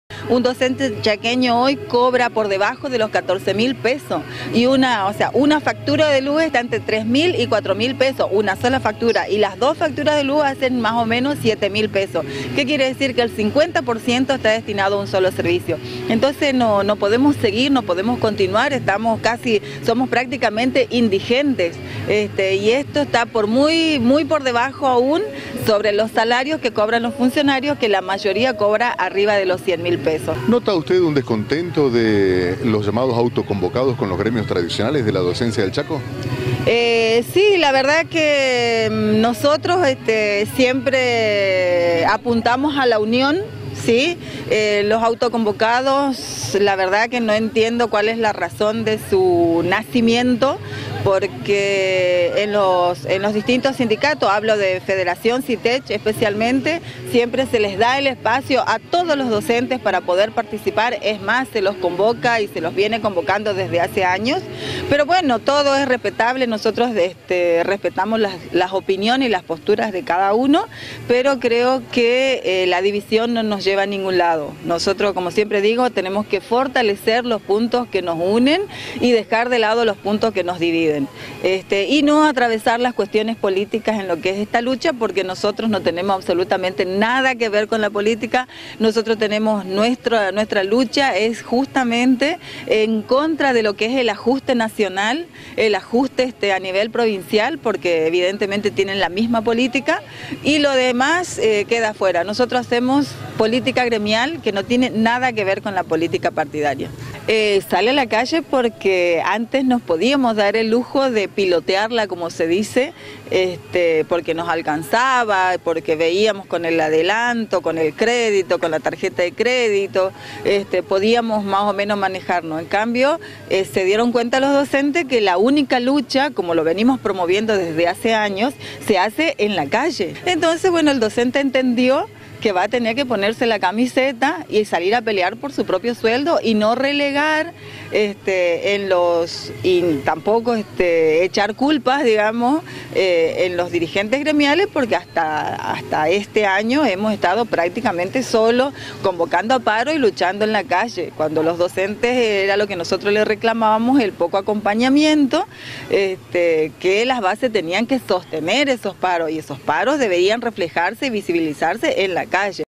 C9 - CHACO; Reclamo docente en Charata
AUTOCONVOCADOS CONTINÚAN MANIFESTÁNDOSE EN LA PROVINCIA
Los docentes de Charata continúan manifestándose en distintos puntos de la ciudad, en la jornada del lunes el punto de encuentro fué la plaza  San Martín, allí una de las docentes se encadenó.
SANDRA HUMAR - FEDERACIÓN SITECH